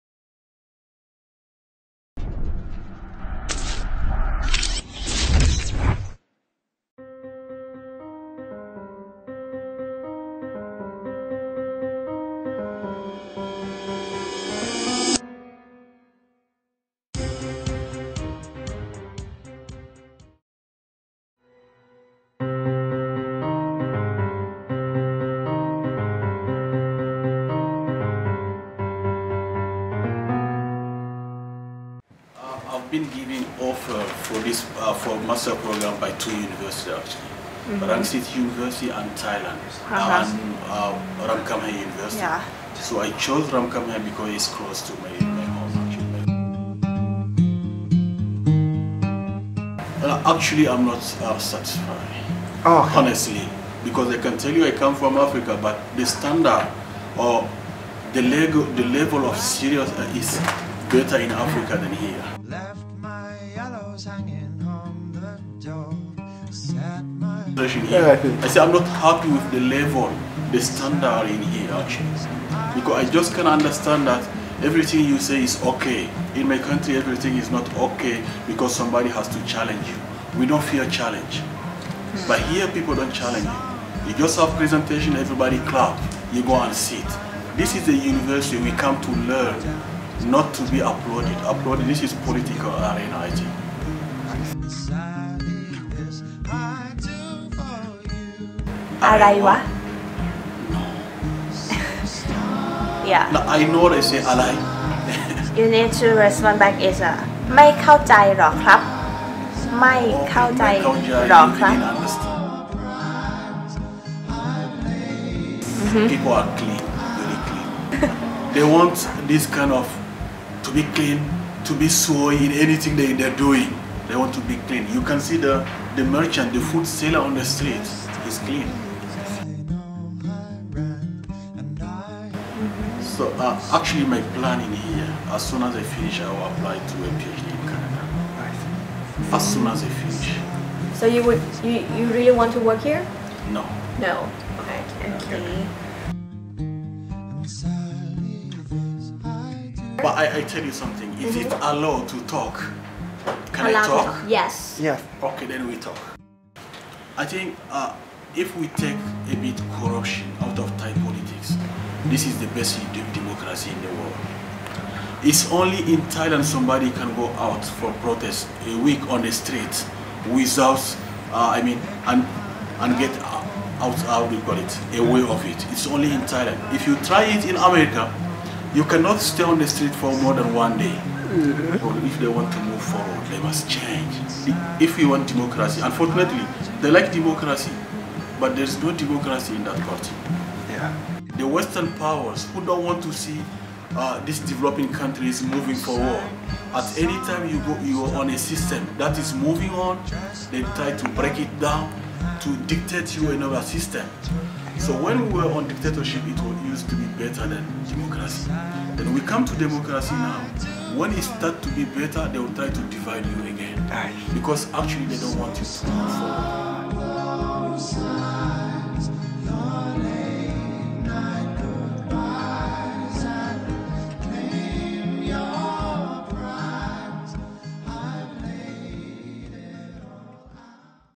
Mr. Diori from Niger Video Interview HD
This video is an assignment required to fulfill in course JR401 IIS-RU

THANAJAK CHUNGVIWATANT
- DIRECTOR / CAMERA

WILASINEE SEMACHAI
- PRODUCER / PROJECT COORDINATOR

SIRIKORN LIANGRAKSA
- CREATIVE DESIGN / EDITOR

PATCHAORN THANEEWATCHARAKUL
- PROJECT COORDINATOR / CO-MODERATOR